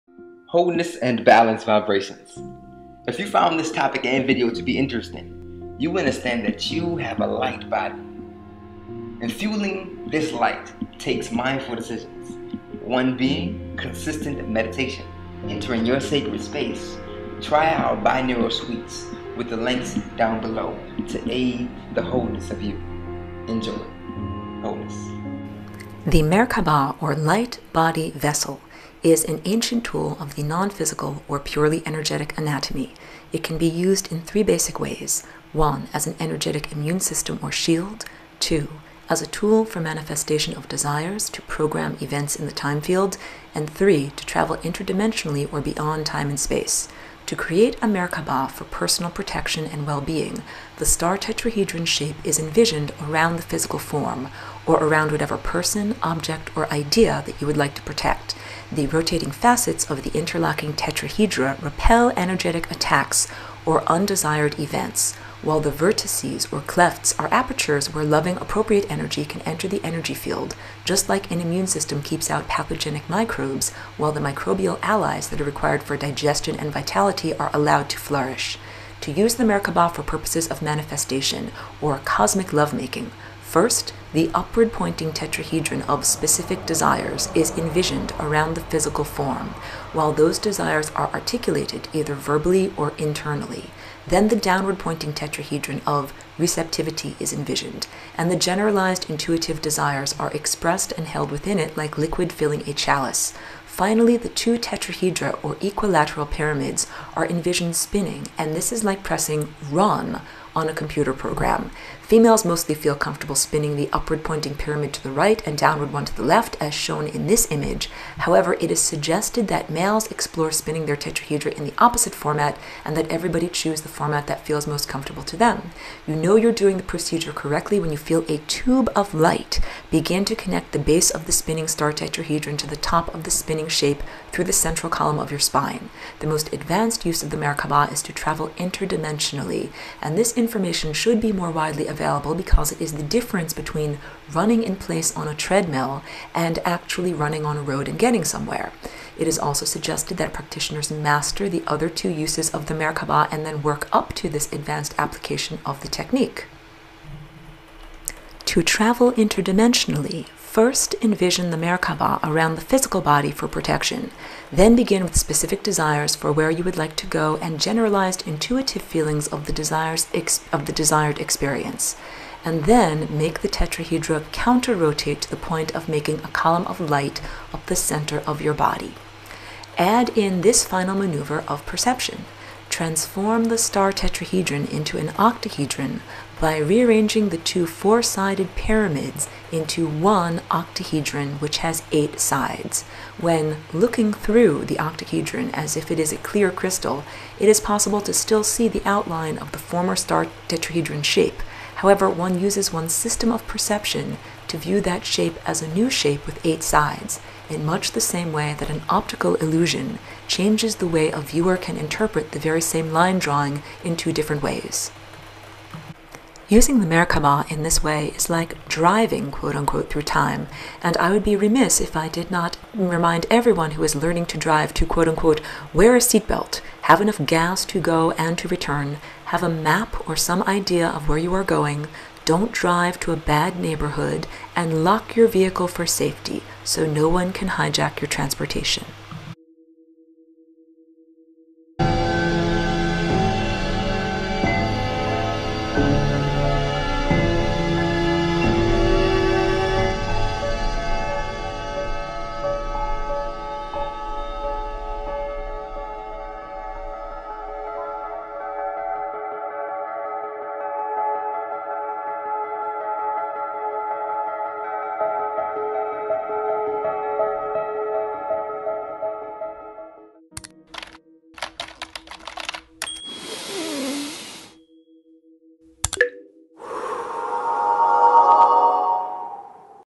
3 ᴛɪᴩꜱ ᴏɴ ɪɴɴᴇʀꜱᴛᴀɴᴅɪɴɢ ᴛʜᴇ ᴍᴇʀᴋᴀʙᴀ (ʙɪɴᴀᴜʀᴀʟ ꜱᴜɪᴛᴇꜱ)
𝙒𝙝𝙤𝙡𝙚𝙣𝙚𝙨𝙨 𝙖𝙣𝙙 𝘽𝙖𝙡𝙖𝙣𝙘𝙚𝙙 𝙑𝙞𝙗𝙧𝙖𝙩𝙞𝙤𝙣𝙨,

𝙎𝙝𝙖𝙧𝙞𝙣𝙜 𝙩𝙝𝙚 𝙇𝙞𝙜𝙝𝙩 𝙖𝙣𝙙 𝙎𝙪𝙘𝙘𝙚𝙨𝙨𝙛𝙪𝙡 𝙒𝙞𝙨𝙙𝙤𝙢 𝙎𝙞𝙨𝙩𝙖𝙧 𝘼𝙪𝙧𝙤𝙧𝙖 𝙨𝙝𝙞𝙣𝙚𝙨 𝙞𝙣𝙨𝙞𝙜𝙝𝙩 𝙩𝙤 𝙩𝙝𝙞𝙨 𝘼𝙣𝙘𝙞𝙚𝙣𝙩 𝙏𝙚𝙘𝙝𝙣𝙤𝙡𝙤𝙜𝙮 𝙤𝙛 𝘼𝙣𝙘𝙚𝙨𝙩𝙤𝙧𝙨. 𝙌𝙪𝙖𝙣𝙩𝙪𝙢 𝙏𝙚𝙘𝙝𝙣𝙤𝙡𝙤𝙜𝙮 𝙩𝙝𝙖𝙩 𝙝𝙖𝙨 𝙗𝙚𝙚𝙣 𝙛𝙤𝙧𝙜𝙤𝙩𝙩𝙚𝙣 𝙩𝙝𝙖𝙩 𝙘𝙖𝙣 𝙗𝙚 𝙪𝙨𝙚𝙙 𝙩𝙤 𝙘𝙧𝙚𝙖𝙩𝙚 𝙖𝙣𝙙 𝙩𝙧𝙖𝙫𝙚𝙡 𝙩𝙤 𝙪𝙣𝙛𝙖𝙩𝙝𝙤𝙢𝙖𝙗𝙡𝙚 𝙙𝙚𝙨𝙞𝙜𝙣𝙖𝙩𝙞𝙤𝙣𝙨.

𝙐𝙨𝙞𝙣𝙜 𝙏𝙤𝙤𝙡𝙨 𝙡𝙞𝙠𝙚 𝙌𝙪𝙖𝙡𝙞𝙩𝙮 𝙁𝙧𝙚𝙦𝙪𝙚𝙣𝙘𝙞𝙚𝙨 𝙩𝙤 𝙖𝙞𝙙 𝙎𝙚𝙡𝙛 𝙞𝙣𝙩𝙤 𝙩𝙝𝙖𝙩 𝙨𝙖𝙘𝙧𝙚𝙙 𝙨𝙥𝙖𝙘𝙚 𝙬𝙞𝙩𝙝𝙞𝙣 𝙩𝙝𝙚 𝙈𝙞𝙣𝙙'𝙨 𝙀𝙮𝙚

𝘿𝙖𝙞𝙡𝙮 𝙎𝙥𝙚𝙘𝙩𝙧𝙪𝙢 𝙍𝙚𝙨𝙤𝙣𝙖𝙣𝙘𝙚: 𝙄𝙣𝙨𝙩𝙖𝙜𝙧𝙖𝙢: @𝘾𝙪𝙧𝙩𝙞𝙨𝙃𝙚𝙧𝙪